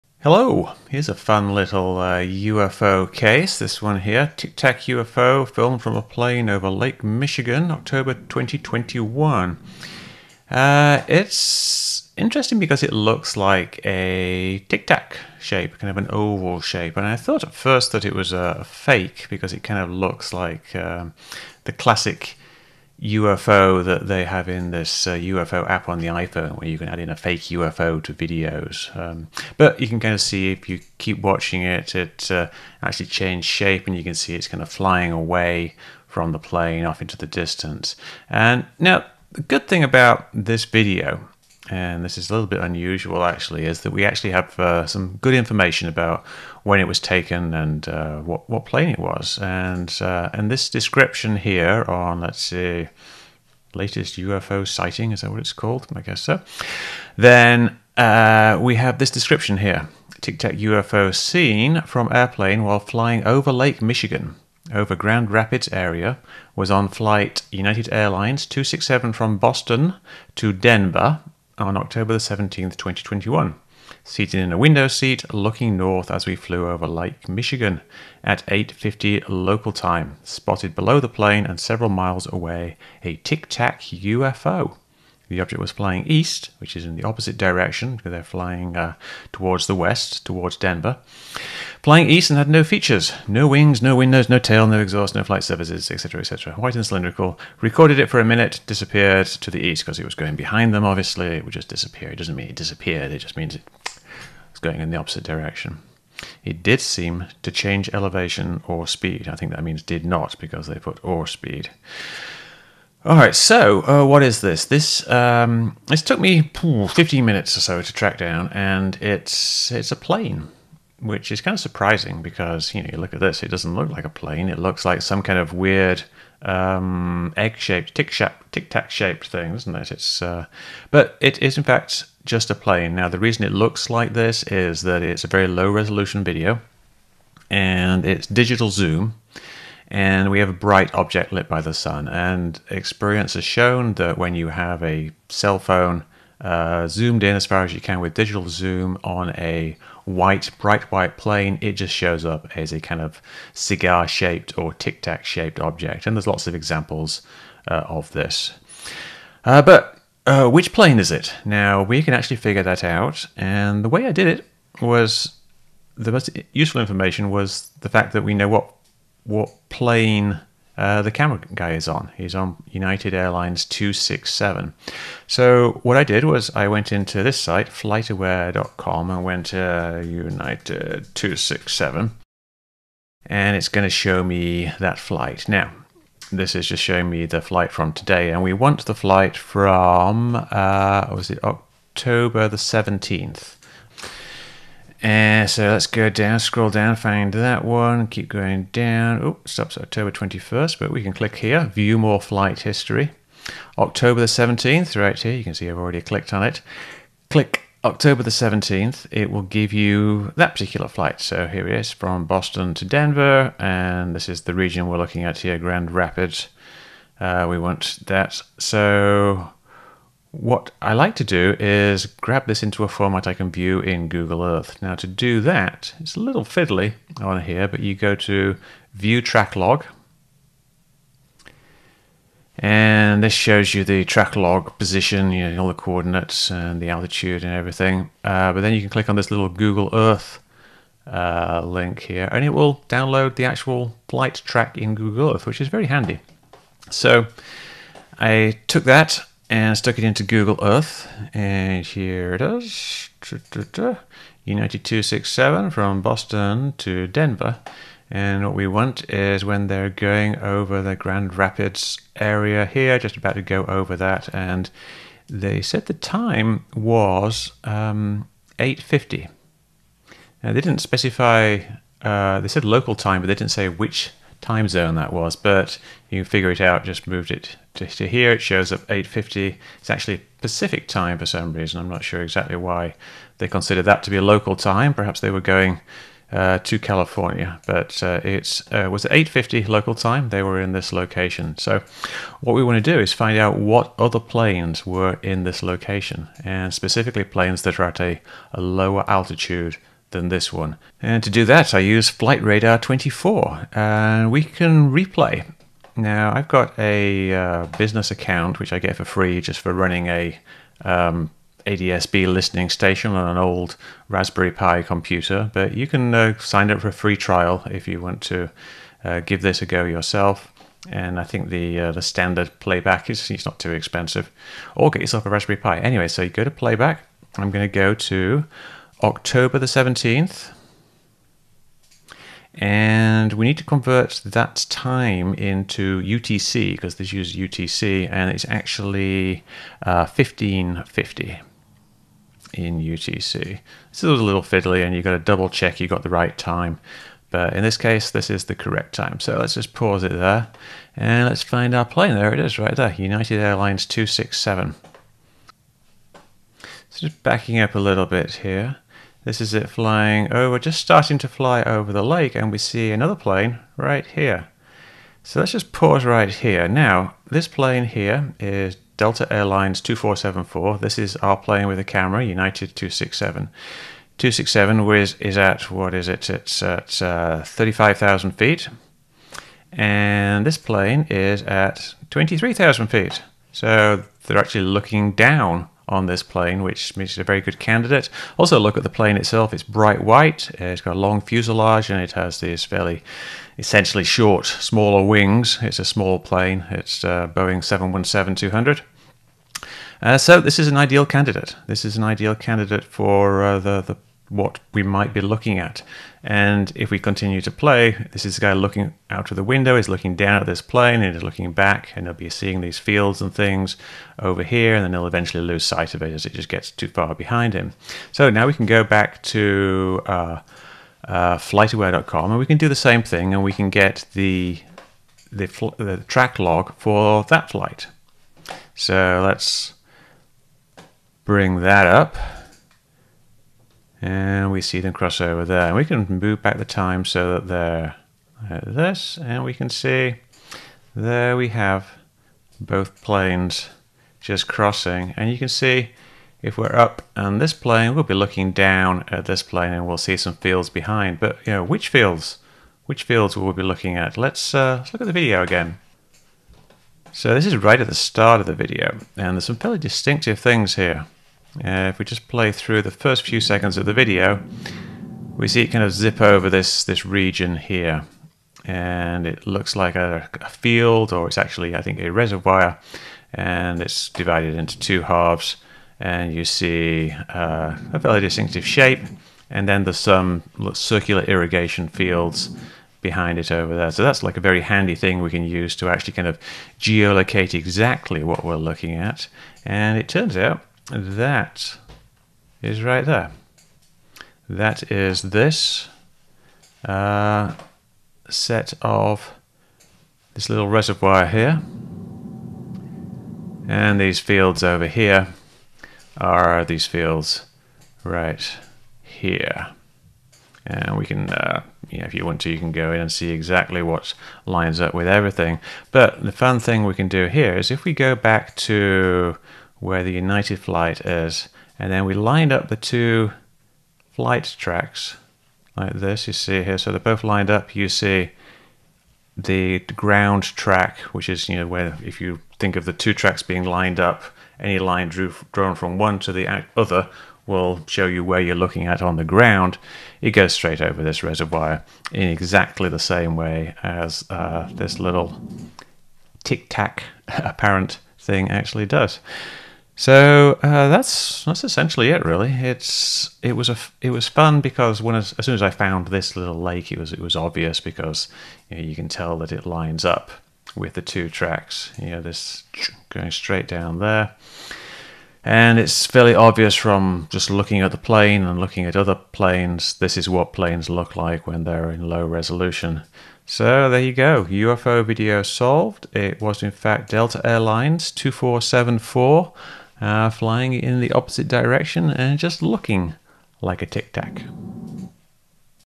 Tracking Down a Tic-Tac UFO
The latest video of a Tic-Tac UFO (no relation to the Navy Tic-Tac) is actually a plane. In this video I show how I tracked down the actual plane and exact location.